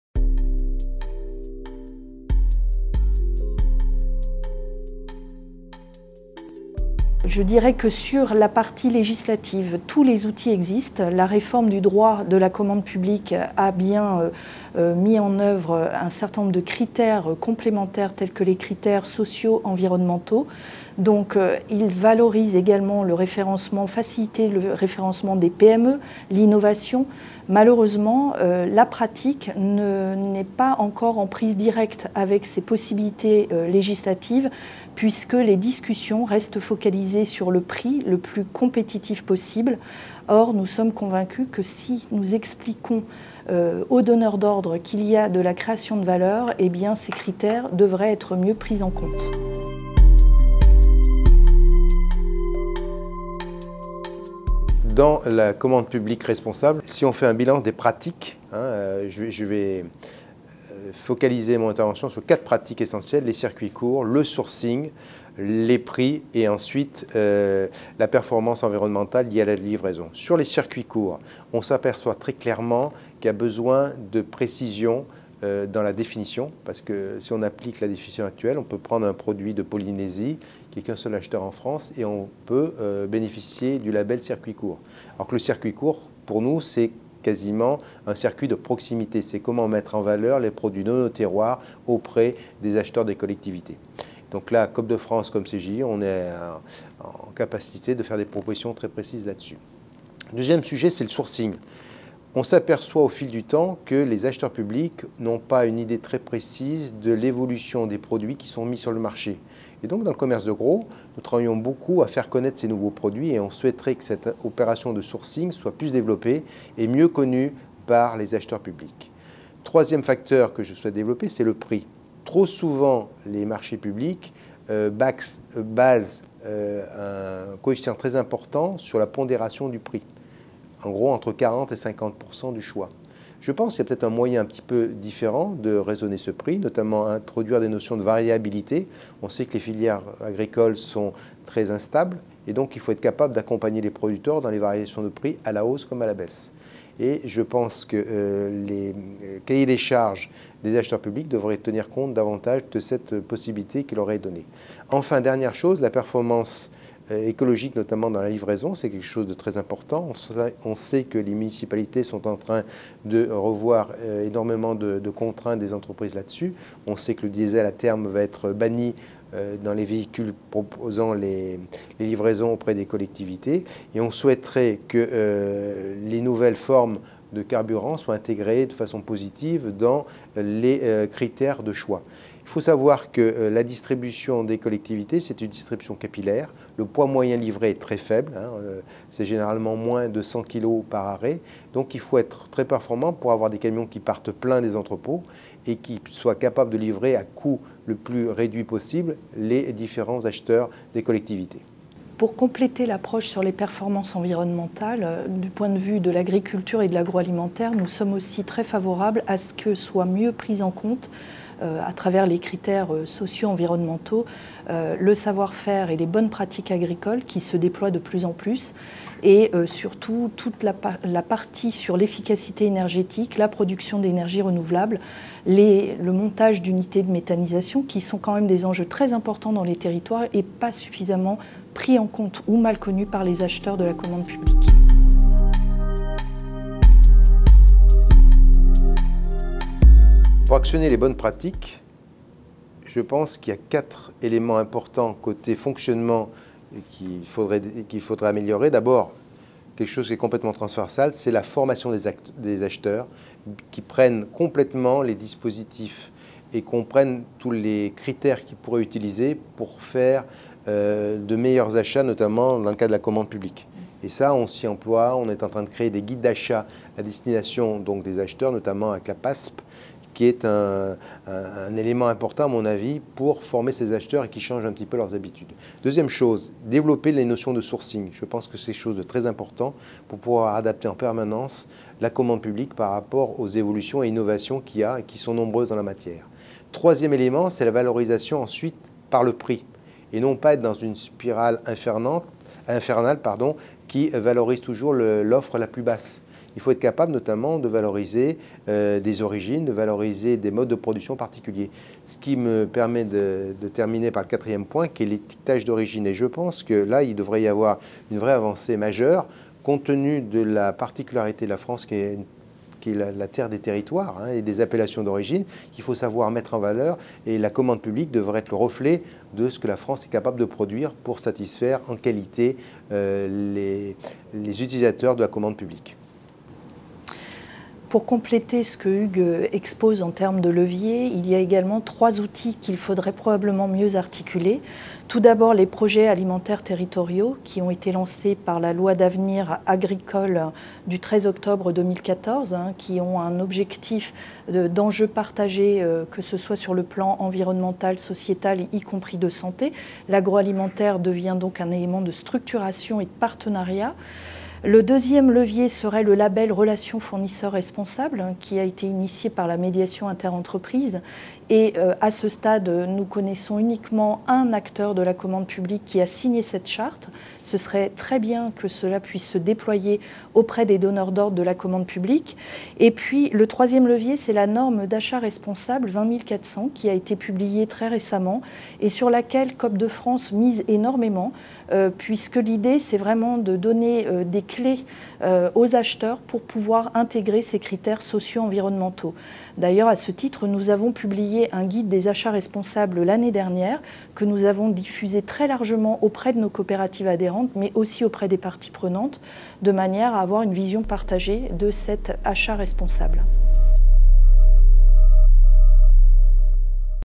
Questions à Rachel BLUMEL et Hugues POUZIN - Commande publique - cese
Questions à  Mme Rachel BLUMEL, directeur Coop de France Agroalimentaire et M. Hugues POUZIN, directeur général Confédération commerce gros et international, auditionnés par la par la Délégation à la prospective et à l'évaluation des politiques publiques du CESE dans le cadre de la saisine : "Commande publique responsable : enjeux/actions".